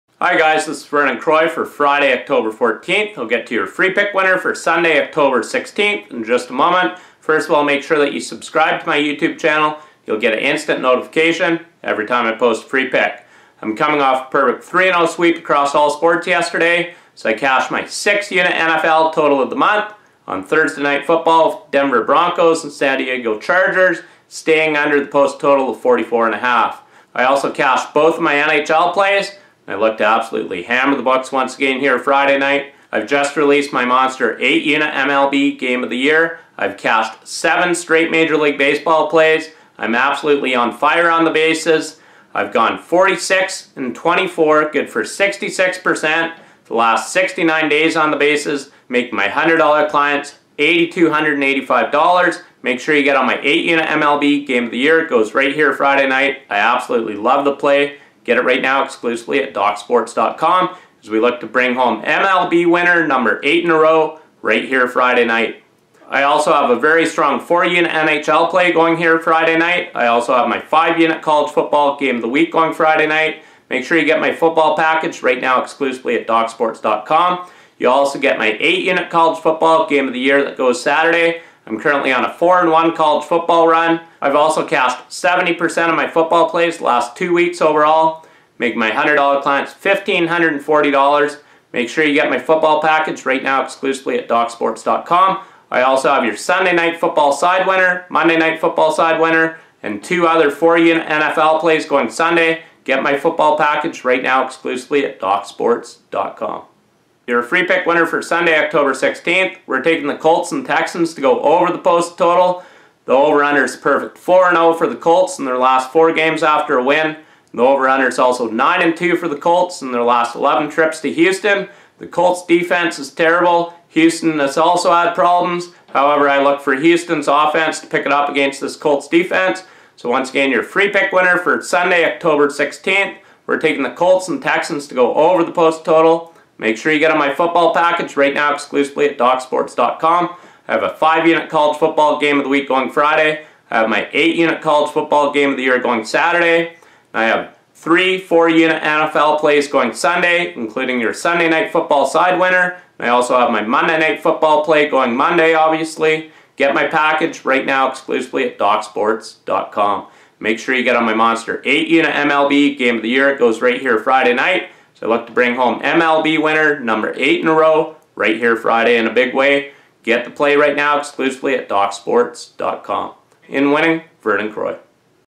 Week 6 Free NFL Picks - Indianapolis Colts vs Houston Texans SNF Prediction 10/16/16 8:30PM ET
Your Week 6 free NFL pick is from handicapping expert Vernon Croy Your free NFL winner for Sunday, October 16, 2016 is between the Indianapolis Colts vs Houston Texans. to get more free NFL predictions each day as well as $60 free of Croy's premium member only predictions. 186-156 free pick video run!